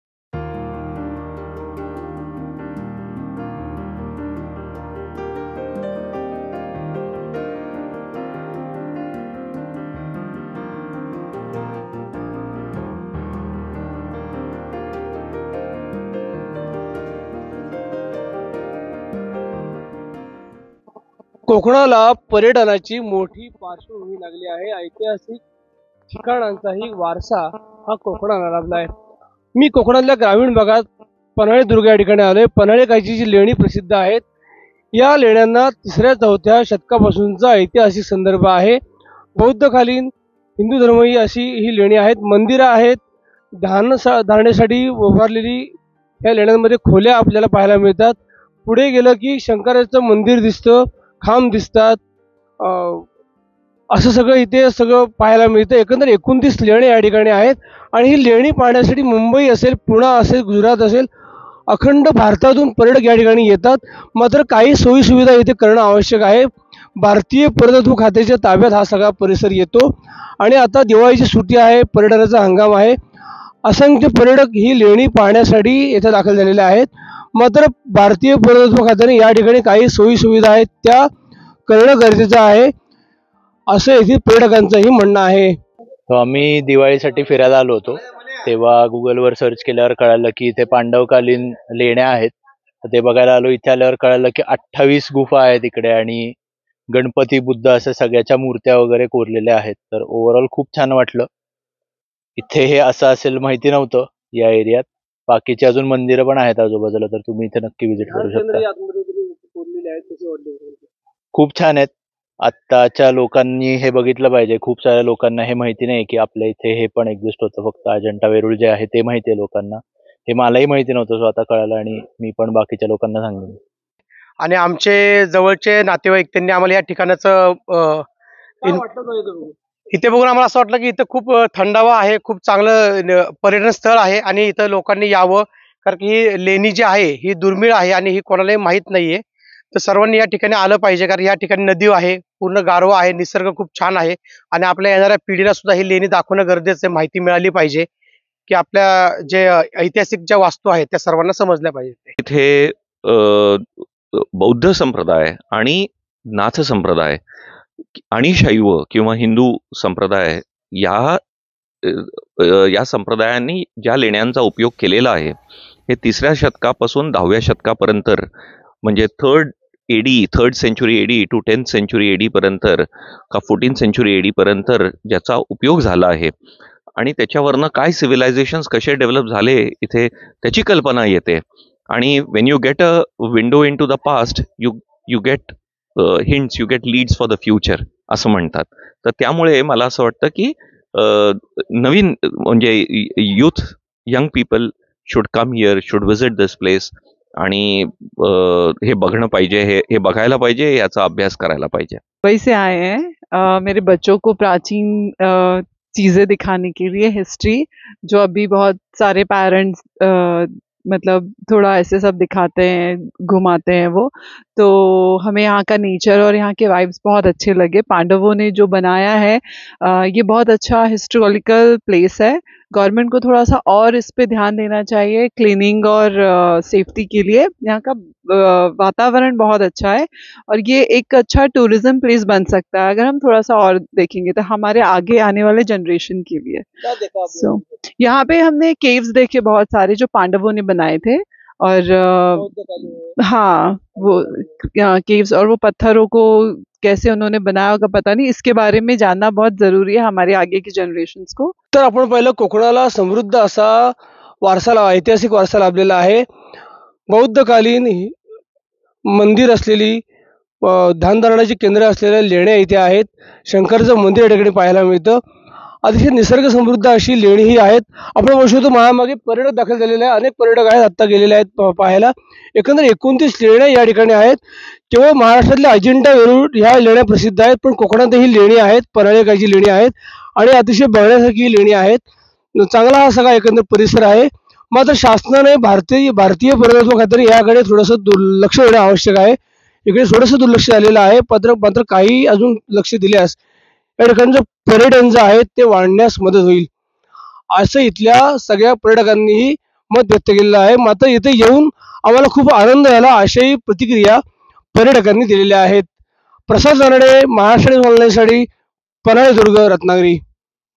Panhale Leni | कोकणात दडलाय समृद्ध लेण्यांचा इतिहास; २९ दुर्मिळा लेण्या पर्यटकांसाठी मोठं आकर्षण
कोकणात रत्नागिरी जिल्ह्यात पन्हाळे दुर्गे येथील लेणी प्रसिद्ध आहेत ही लेणी पाहण्यासाठी.अवघ्या महाराष्ट्रासह राज्याबाहेरील पर्यटकांनीही या मोठी गर्दी केली आहे. या लेण्यांच्या इतिहासाचा संदर्भ हा थेट तिसऱ्या, चौथ्या शतकापर्यंत जातो.सह्याद्री पर्वतरांगेच्या पायथ्याशी कोटजाई व धाकटी नदीच्या खोऱ्यात हे स्थळ आहे. साधारणतः इ. स. तिसऱ्या शतकापासून ते चौथ्या शतकापर्यंत समृद्ध अशा इतिहासाची साक्ष असणाऱ्या.पांडवकालीन एकूण २९ दुर्मिळा लेण्या खोदण्यात आल्या आहेत. यातील २८ लेण्या या कोडजाई नदीच्या उजव्या किनाऱ्यावर उत्तराभिमुख असून २९ वे लेणे. बागवाडीजवळ 'गौर लेणे' या नावाने प्रसिद्ध आहे. हीनयान (थेरवाद), वज्रयान, शैव, गाणपत्य व नाथ संप्रदायाचा ठसा या लेण्यांवर पडलेला दिसून येतो.१९७२ च्या आसपास या पुरातन लेण्यांचा शोध लागला असून ही लेणी डोंगरात लुप्त होती.समृद्ध निसर्ग संपदा, दूरवर पसरलेली आमराई, एका बाजूला असलेली नदी व किनाऱ्यावर असलेली.. ही लेणी पाहण्यासाठी पर्यटक इथे मोठी गर्दी करतात.